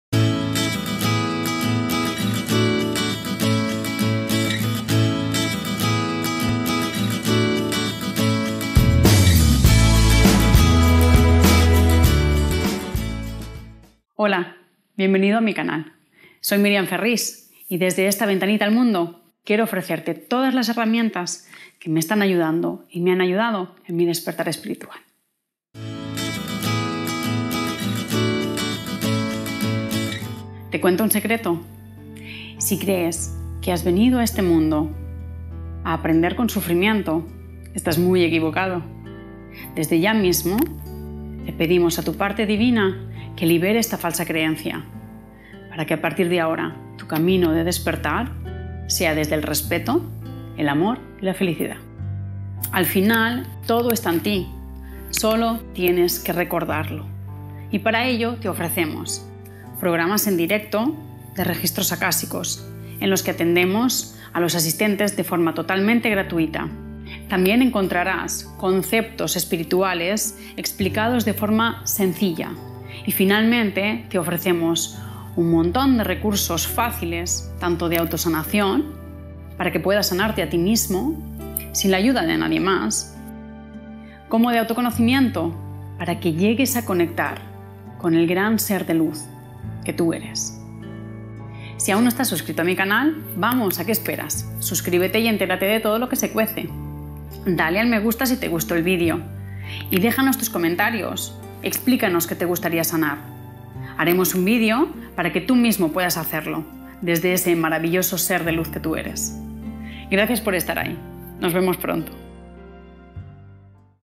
Bienvenidos al canal de Miryam Ferris - Registros Akáshicos
Aquí encontrarás todo aquello que me ayudó y me sigue ayudando en mi despertar espiritual. Todas las herramientas que yo uso las tienes disponibles en este espacio:
* Sanaciones desde los Registros Akáshicos en directo
* Meditaciones
* Explicaciones Sencillas de conceptos que pueden parecer complicados
* Cómo conectar con Seres de Luz
* Ejemplos de sesiones de Registros Akáshicos
* Videos que te ayudarán en tu autoconocimiento
* Cómo sanarte a ti mismo

Todo ello para que puedas conectar con el gran ser de Luz que eres y puedas vivir desde la felicidad y amor que hay en tí.
Viviendo en la Tierra 2.0,  siendo un sitio idílico para estar y desarrollarse.

Un abrazo de Luz,
Miryam Ferrís

👍 AGRADECIMIENTOS

PRODUCTOR - Matias Campos

¡Gracias!
Miryam Ferris _________________________________